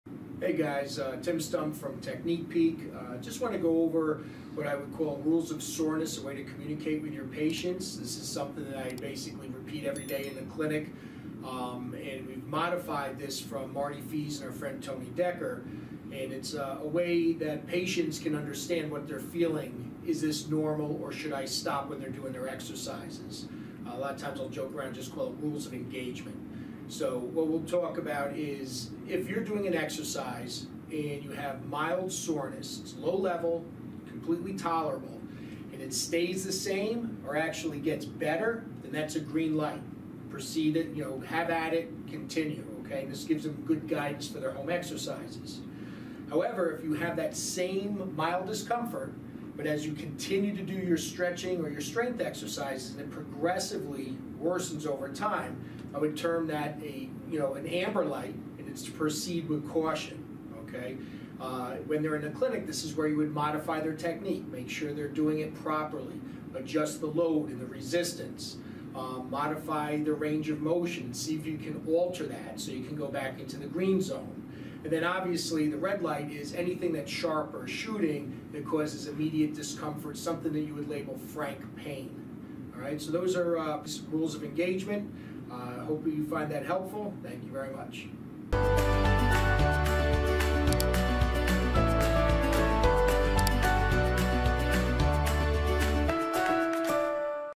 BEST Way to Progress Your Patients Exercise Load for ULTIMATE Results | Technique Peek Series
This Technique Peek Series video features Timothy Stump, MS, PT, CSCS, discussing the Rules of Soreness, which can be used to properly progress your patients' exercise loads. This is proven systematic approach to progressive loading.